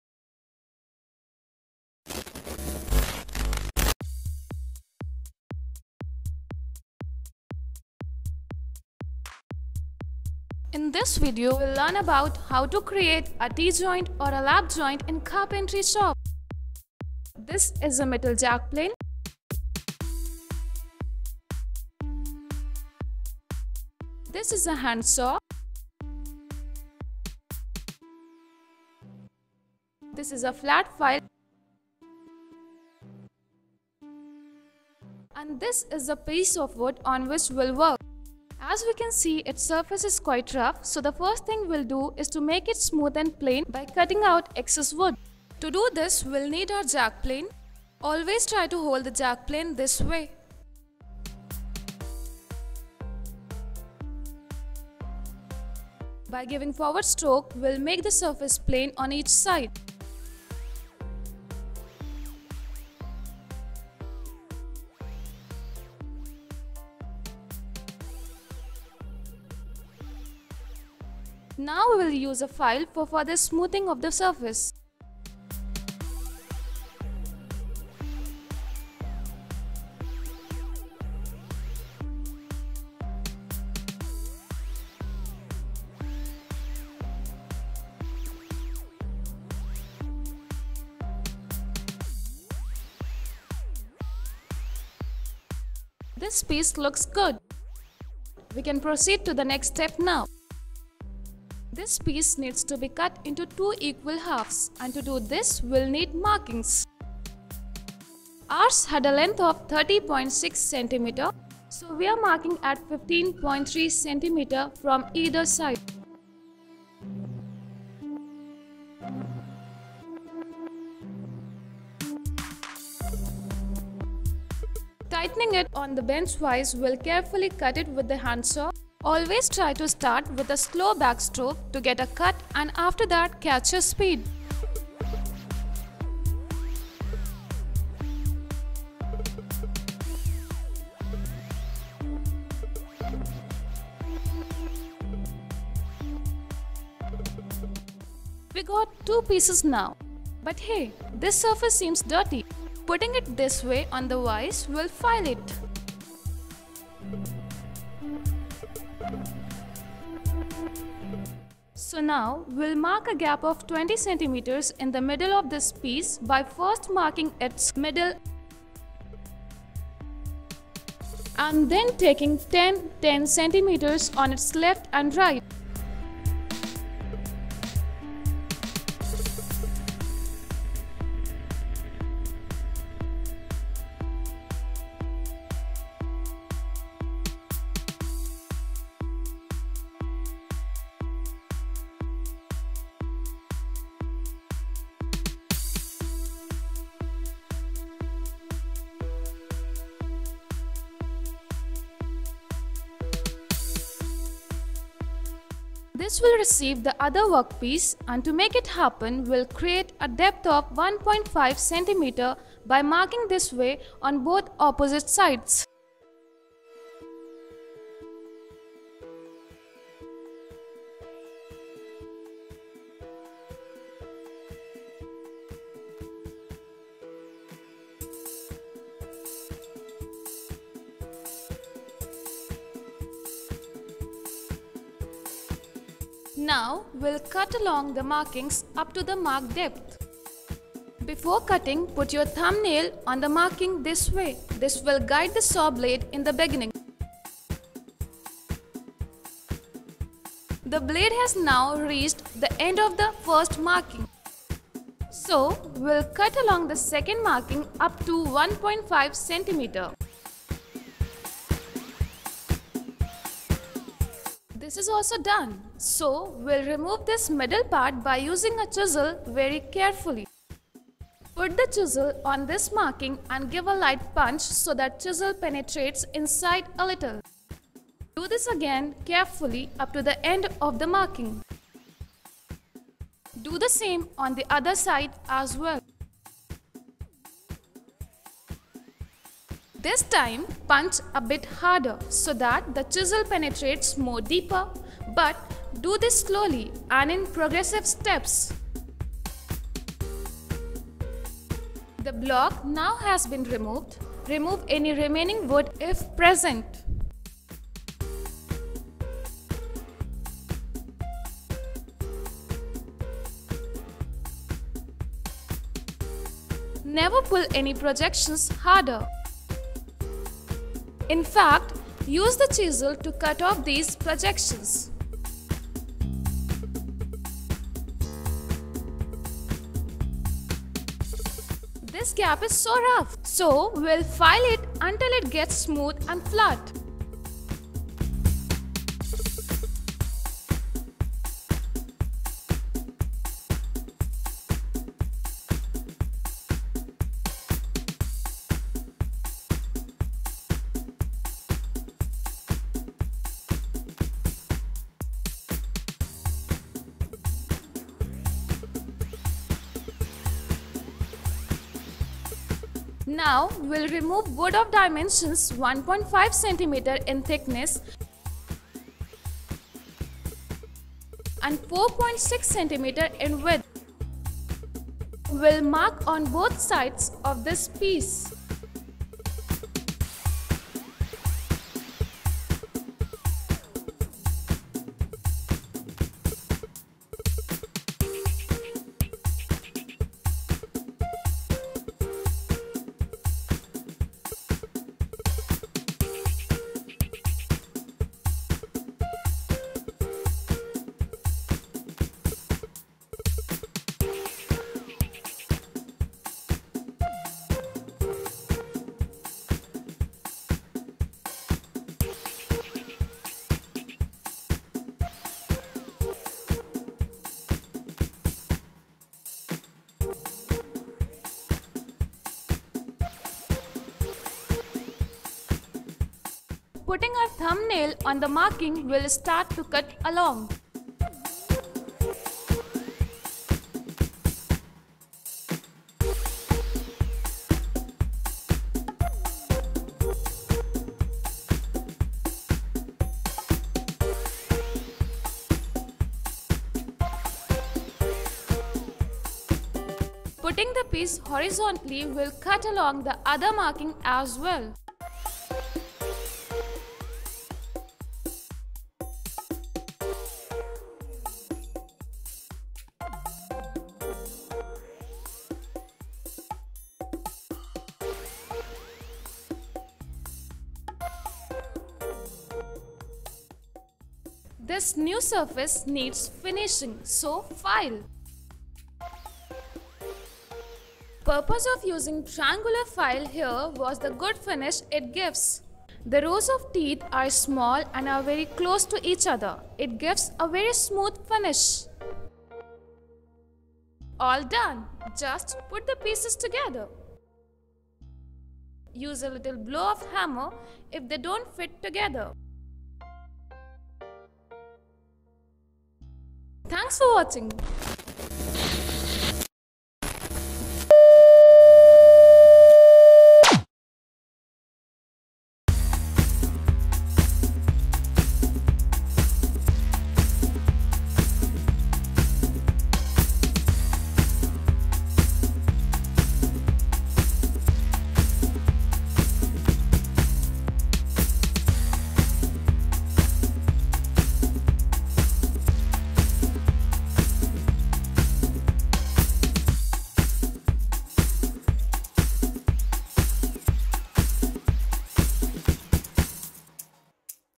How to create a T joint/Lap joint in carpentry shop
About to make a lap joint but thinking how to do that?
Then you are at the right place.We'll tell you which saw to use,how to cut the wood,give a good finish to it using jack-plane and files,along with some good tips!